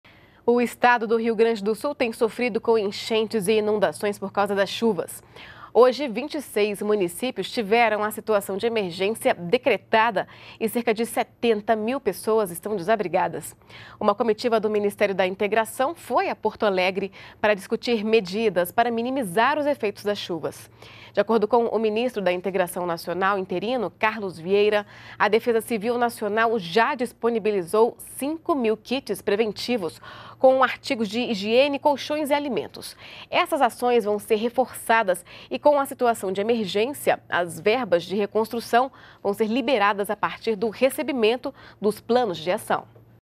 Reconhecida a situação de emergência em 26 municípios do Rio Grande do Sul
NBR NOTÍCIAS - 15.10.15: A Secretaria Nacional de Proteção e Defesa Civil (Sedec) do Ministério da Integração Nacional reconheceu nesta quinta-feira (15) situação de emergência em 26 municípios do Rio Grande do Sul. Cerca de 70 mil pessoas estão desabrigadas por causa das chuvas intensas, granizos, enxurradas e inundações. A Defesa Civil disponibilizou 5 mil kits de ajuda humanitária para auxiliar as vítimas das chuvas no estado.